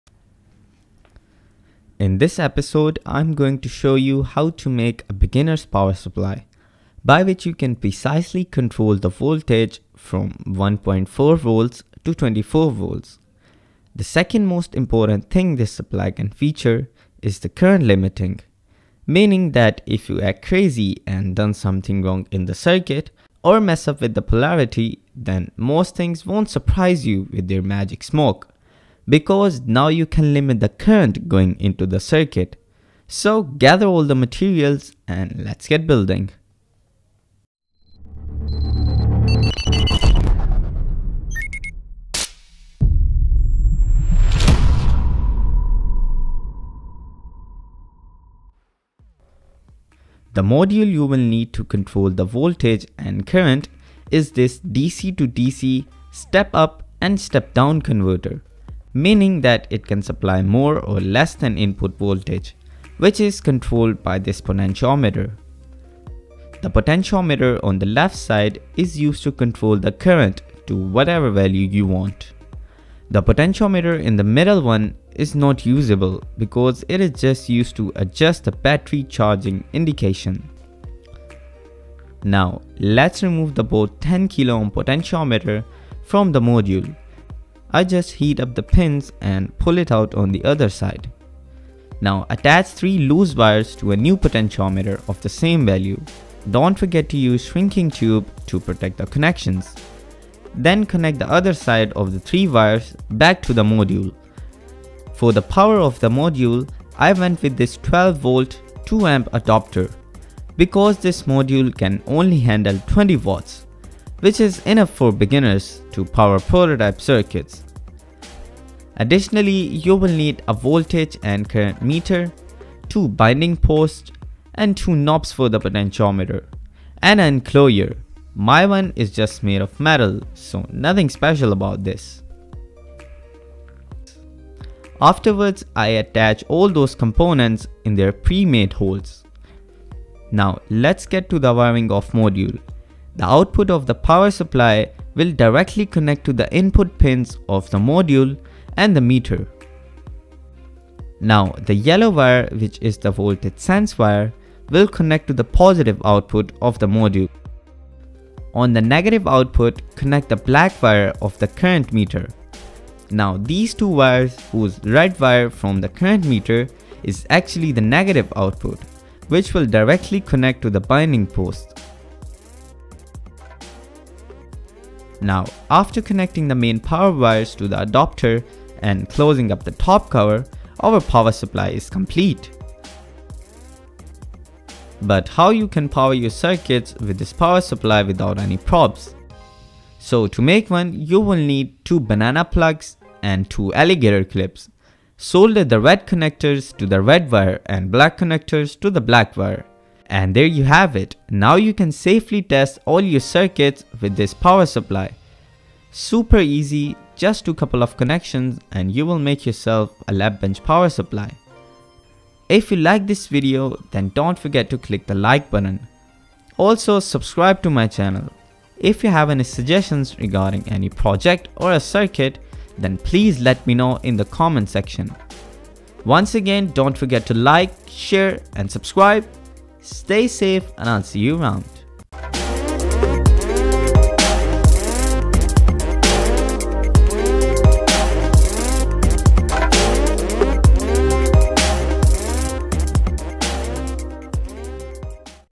How To Make Lab Bench Power Supply || Lab Power Supply For Beginners
In this tutorial i will explain you step by step how to make a lab bench power supply .This tutorial is especially for beginners because you only have to do couple of connection to make this power supply basically its all modular

1) buckboost converter
2)voltage and current meter
3)power supply 12v , 2a
4)10kohm potentiometer and knobs (2x)
5)binding posts
6)alligator clips
7)banana plugs